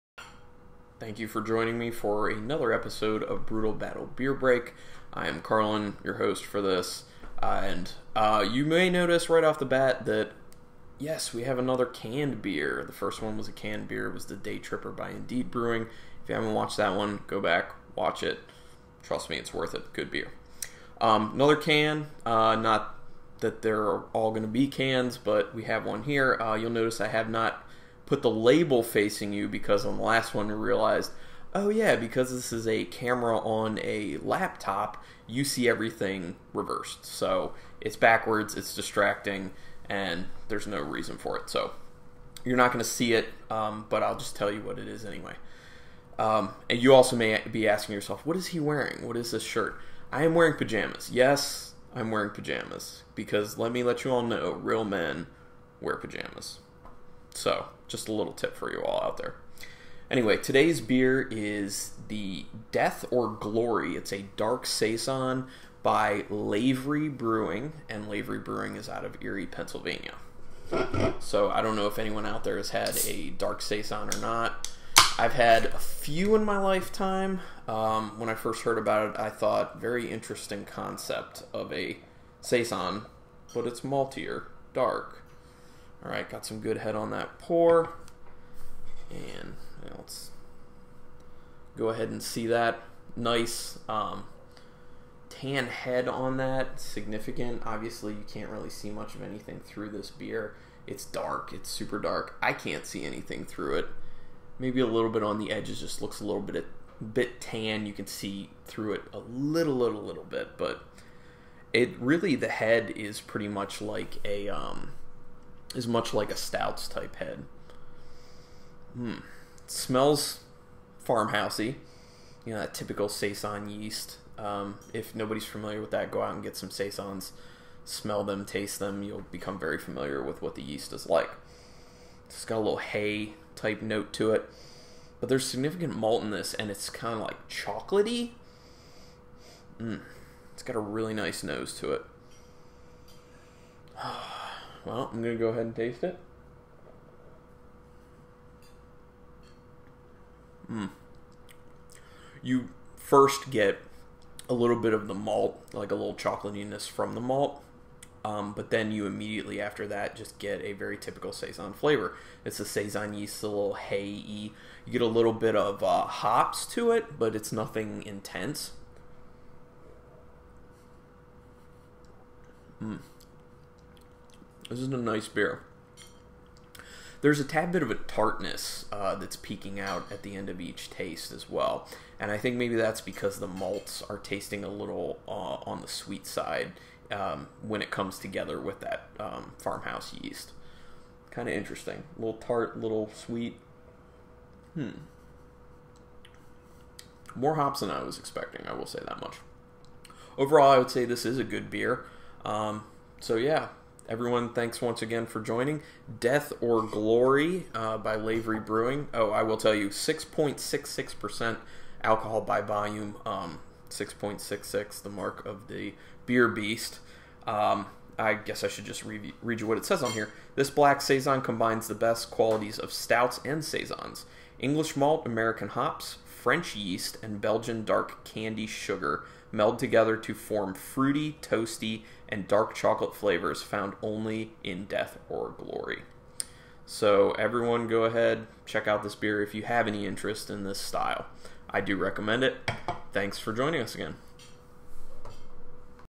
Brewtal Battle Beer Break - Death Or Glory by Lavery Brewing (Ep. 2)
Carlin reviews Death or Glory, a black saison by Lavery Brewing.